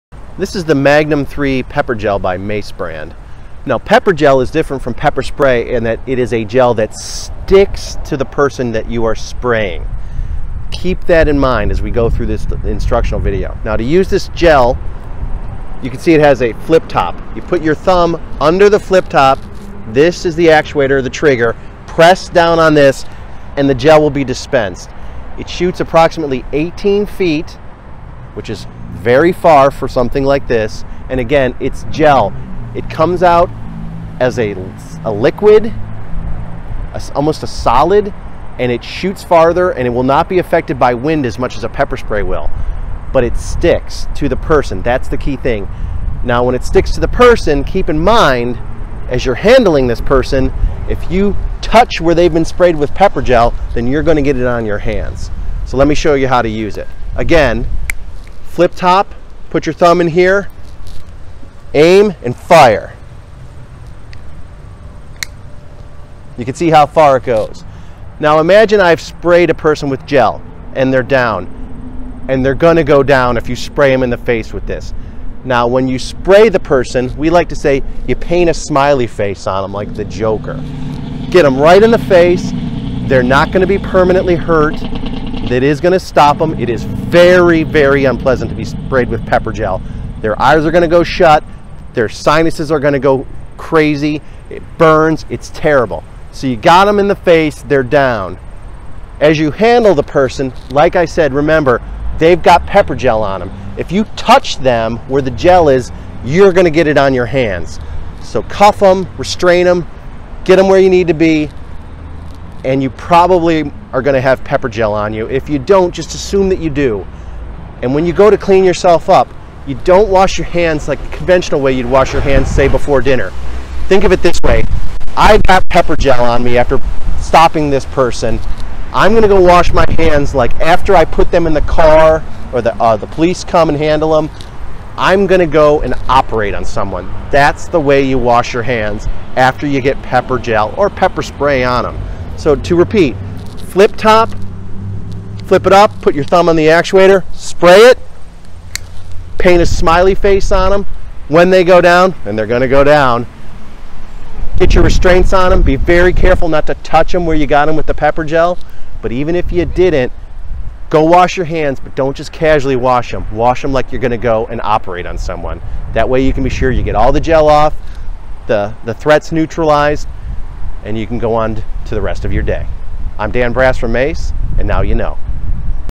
Magnum 3 Pepper Gel instructional video - Mace® Brand
Director of Sales Dan Brass shows you how to use the Mace® Brand Magnum 3 Pepper Gel.
This Mace® gel spray is powered by the hottest pepper allowed by law.
Shoots the furthest distance in its category.
Sticks to the face of your attacker.
Self defense that sticks
You don't have to worry about wind blowback or misfires. Thanks to gel stream technology, the Mace® Magnum 3 pepper gel allows you to quickly hit the mark and protect yourself against threats from a distance.
• Gel stream technology dispenses as a thick, glue-like substance that sticks to and incapacitates the attacker
• OC pepper gel causes respiratory distress and coughing, impaired vision and an intense burning sensation to the skin
• UV dye leaves a long-lasting residue to support investigation and identification
• This Mace® gel spray contains 20 bursts with a spray range of 18 feet, but always replace use.
Ideal for vehicle defense. Clip it to your sun visor for protection from carjackings and other in-vehicle dangers.
Enhanced control and comfort

Compact and easy to carry, all you have to do is grab your Mace® gel spray and go. And, thanks to reliable safety controls, there’s no need to stress about accidental misfires.

• Flip-top safety cap to prevent accidental discharge
• Built-in belt clip for fast and easy access
Always ready

Pepper Gel Magnum 3 is ready if and when you need it. Simply put your thumb under the flip-top safety cap and press down on the actuator button to spray.